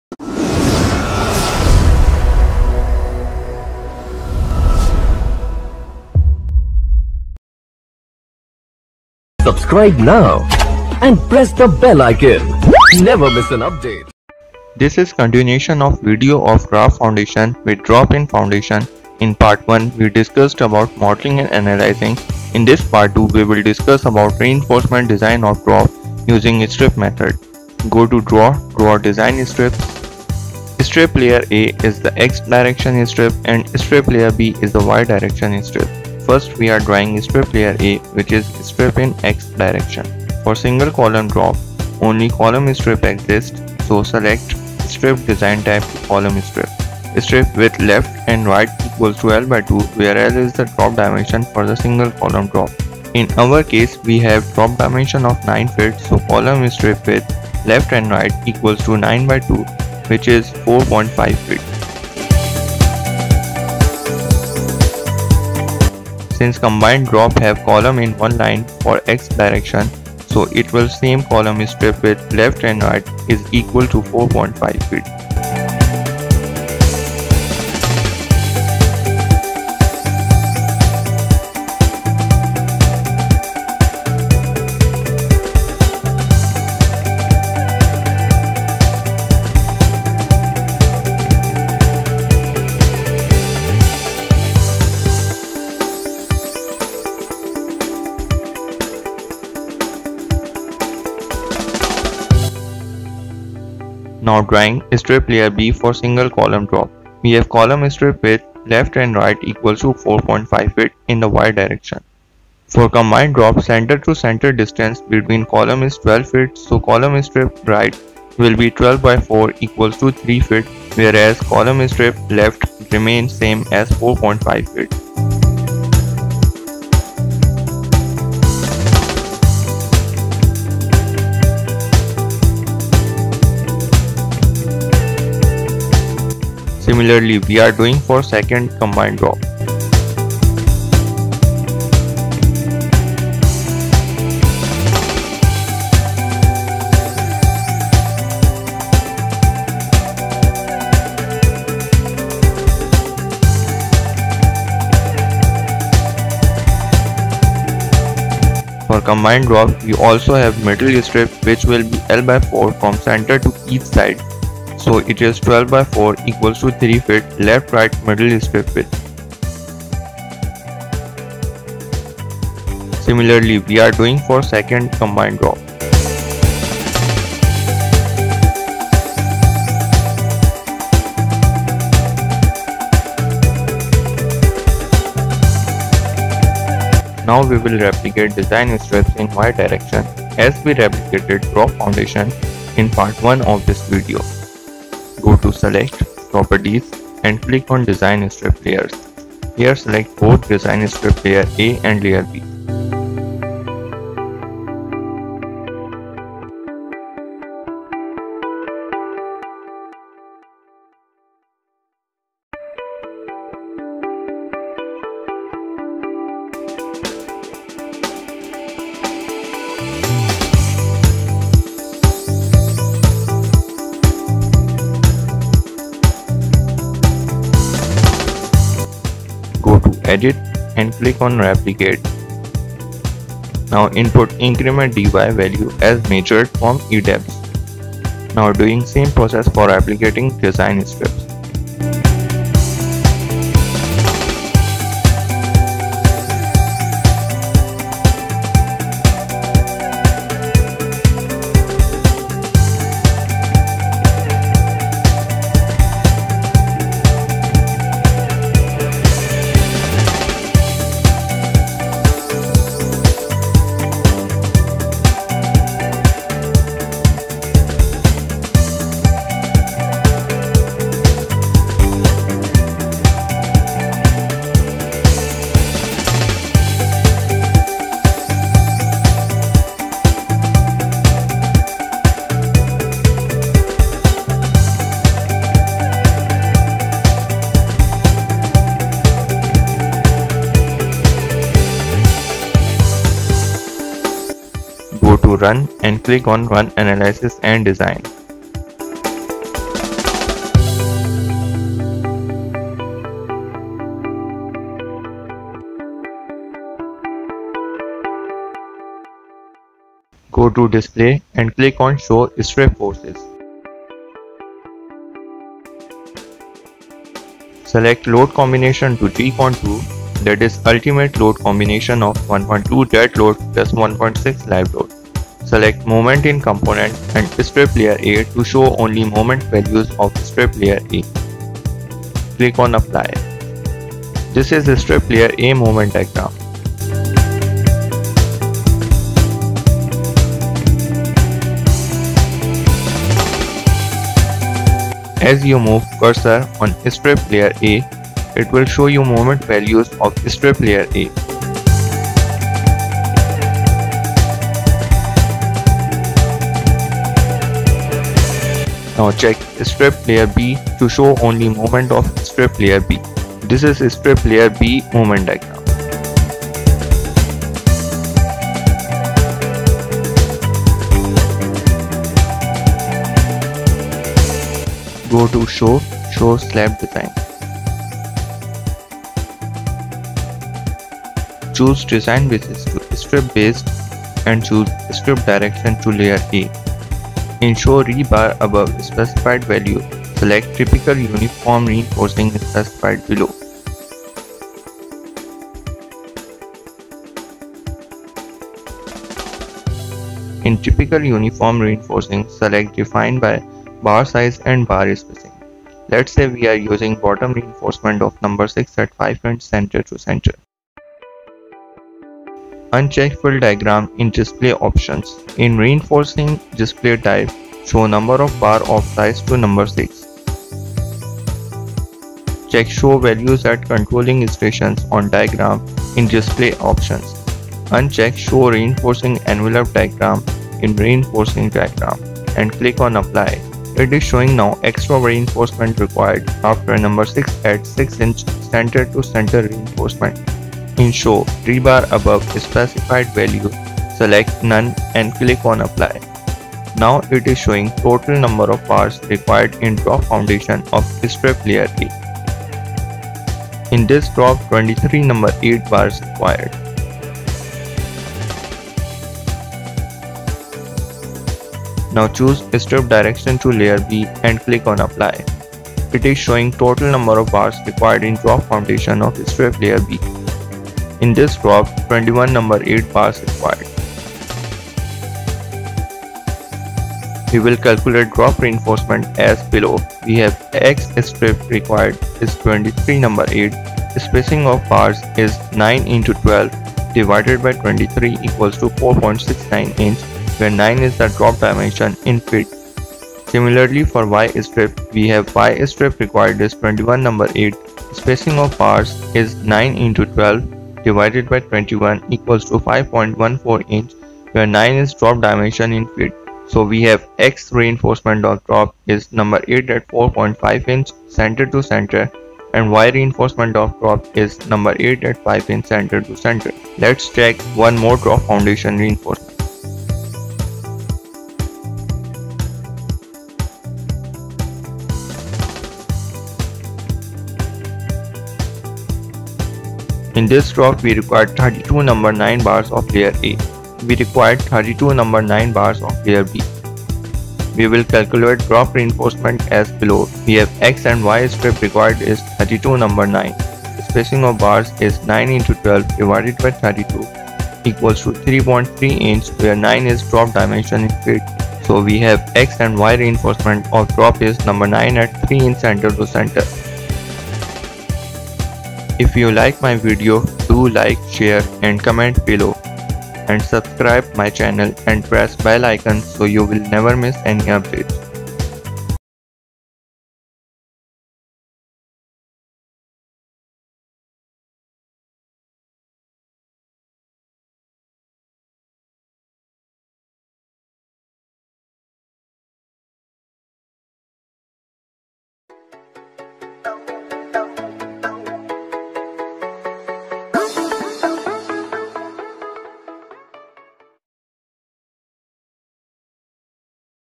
RAFT/MAT Foundation With Drop in SAFE v12 (Part-2) Reinforcement of Drop using Strip Method
(1) Draw Design Strips @ 00:27 (2) Strip Forces @ 05:09 (3) Strip Method Reinforcement Design @ 06:15 (4) Drop Reinforcement Calculation @ 07:31